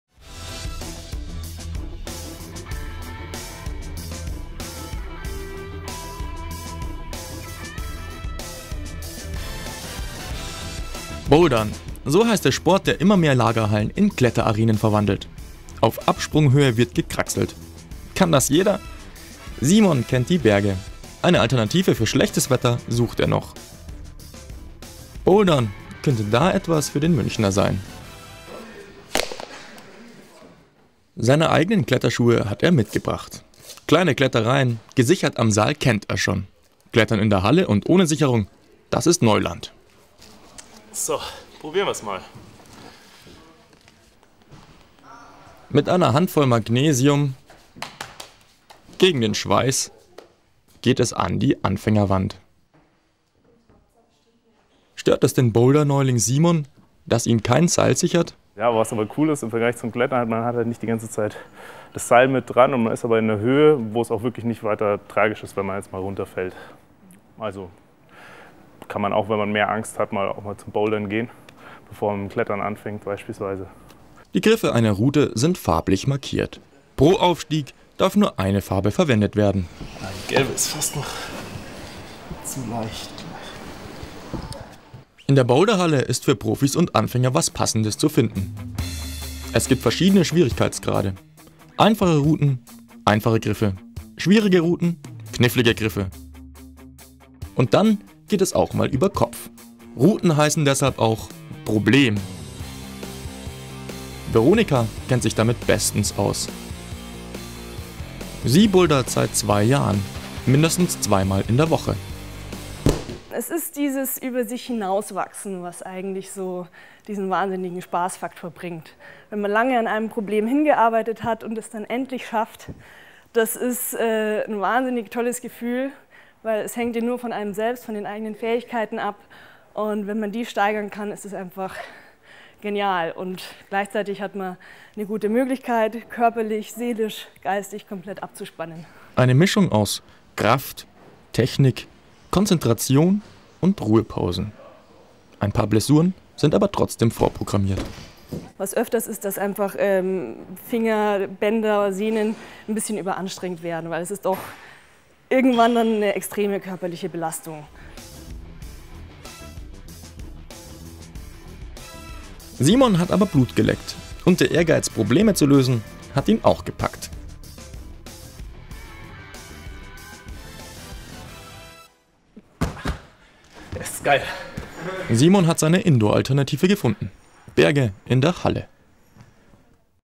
Faszination Bouldern
Ein Besuch in der zur Zeit weltgrößten Boulderhalle in München soll helfen, der Faszination hinter dem Trendsport Bouldern auf den Grund zu gehen. Das Film zeigt Simon Zehetmayer, einen begeisterten Outdoor-Sportler, bei seinen ersten Boulder-Versuchen. Veronika Holler, die seit zwei Jahren bouldert, erklärt unter anderem den Spaßfaktor. Zu sehen sind spektakuläre Kletteraktionen.
Benedikt Angermeier